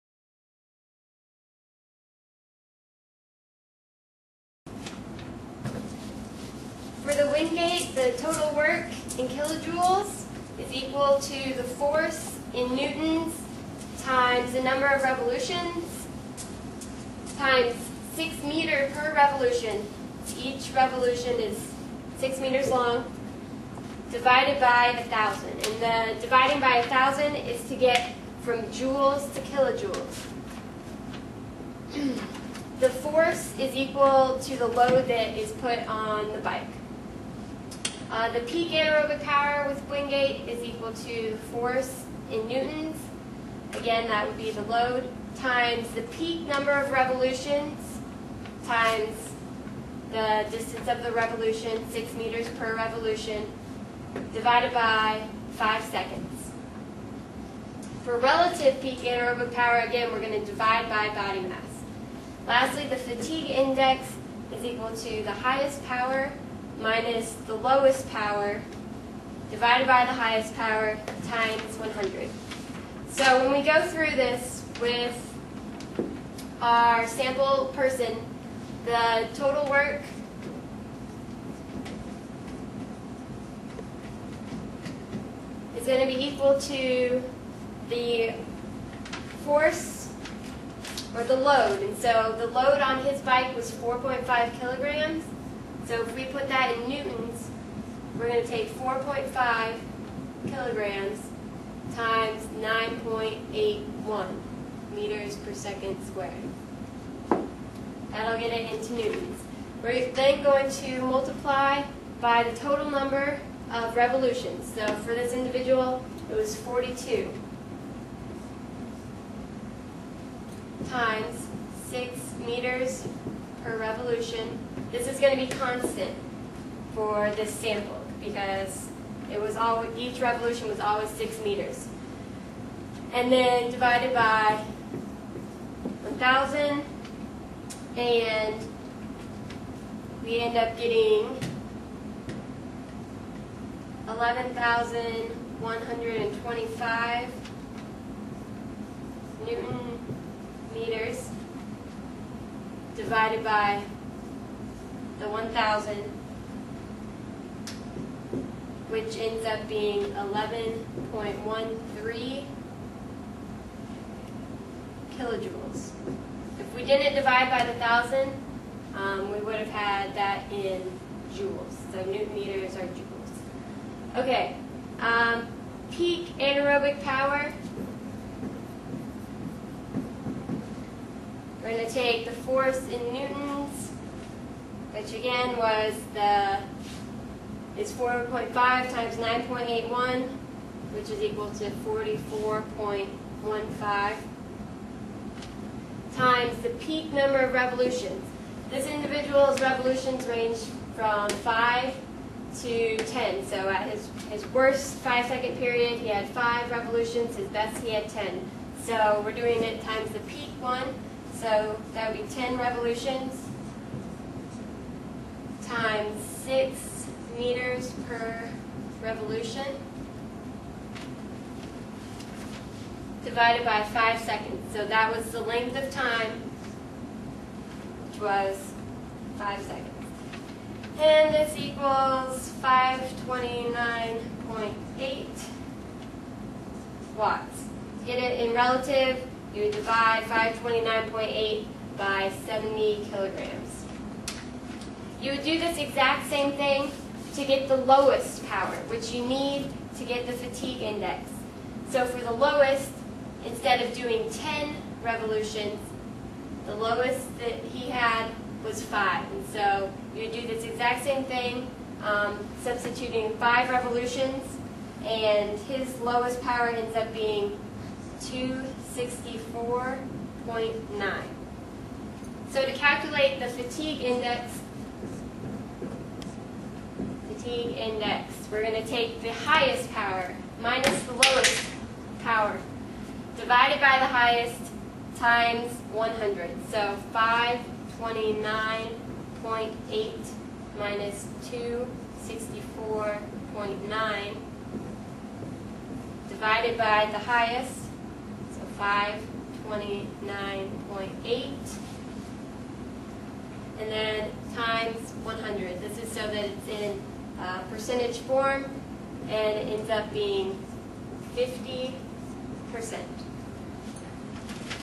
Anaerobic Wingate
Sample calculations for the anaerobic power and capacity lab -- for the Wingate test only.  **Margaria-Kalamen and vertical jump are on a separate video**